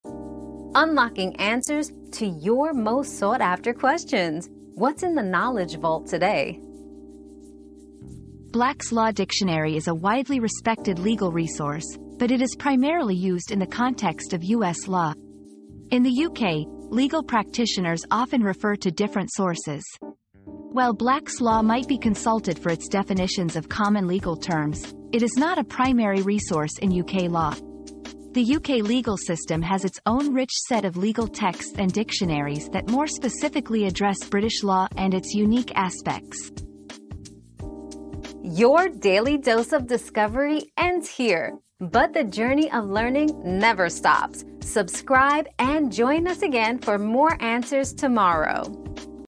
Is black's law used in the UK?
Is Black's Law Used in the UK? 👉 Black's Law in the UK? 👉 Discover whether Black's Law Dictionary, a renowned legal resource, is commonly used in the UK legal system. Find out how the UK legal practitioners refer to different sources and explore the unique aspects of British law.

Laura S. Harris (2024, January 23.) Is black's law used in the UK?
    🌐 AskAbout.video